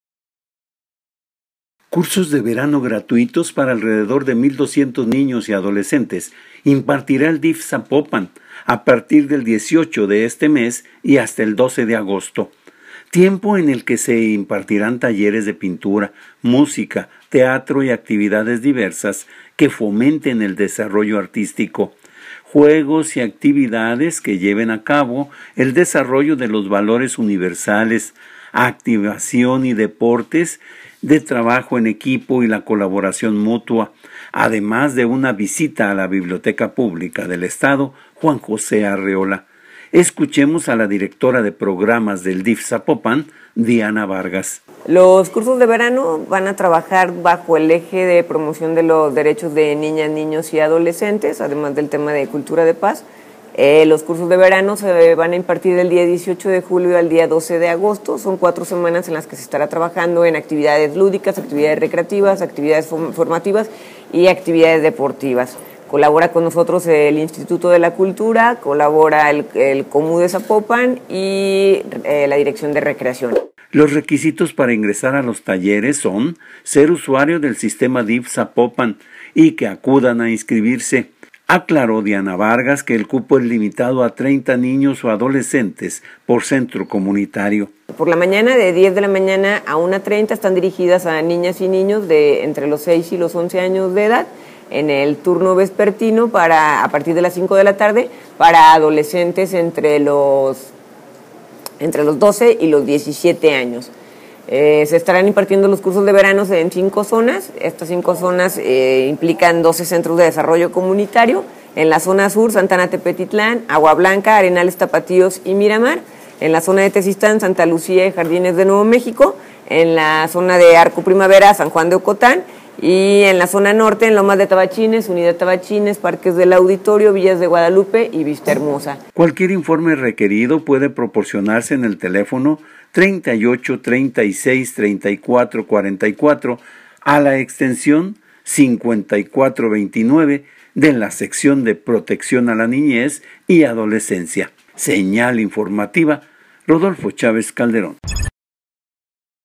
Prepara el DIF Zapopan cursos de verano
El cupo es para mil 200 niños y adolescentes.